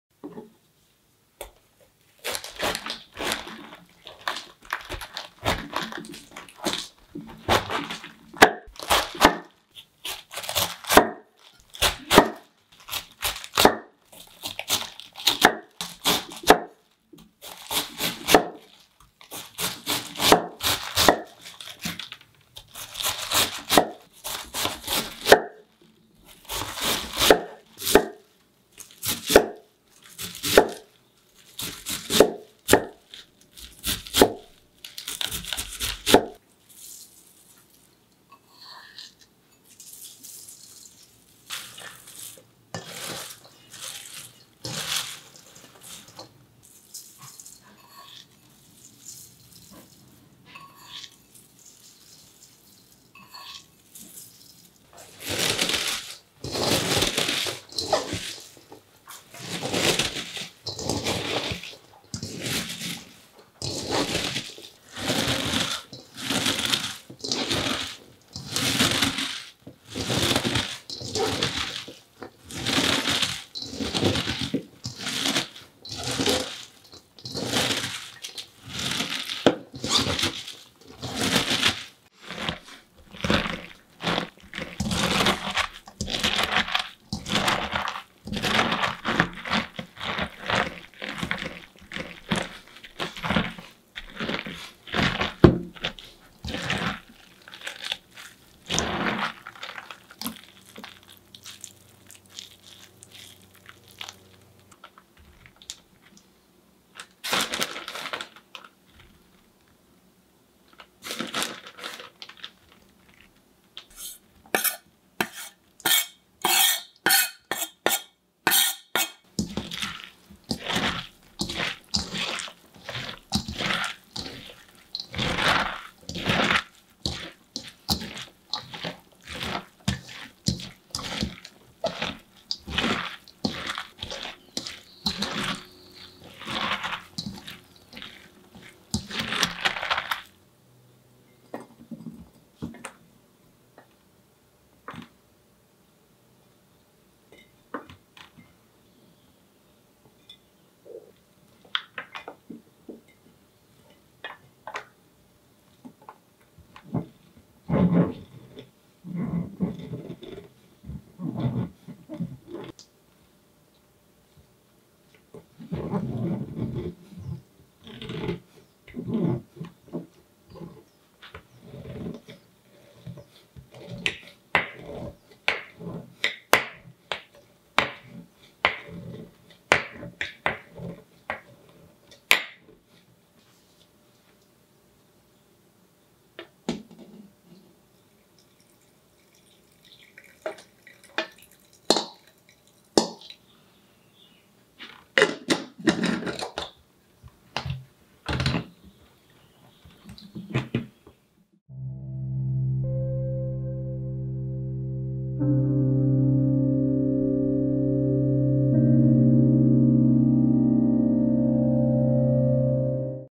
Making Sauerkraut | Chopping Cabbage | ASMR
This is the first time i've made Sauerkraut. Hope it turns out good.

I weighed my cabbage to determine the amount of salt to add.
This is a 2% salt mixture.

I added carrots and garlic (this is optional)

used an Easy Fermenter Lid

Shun Knife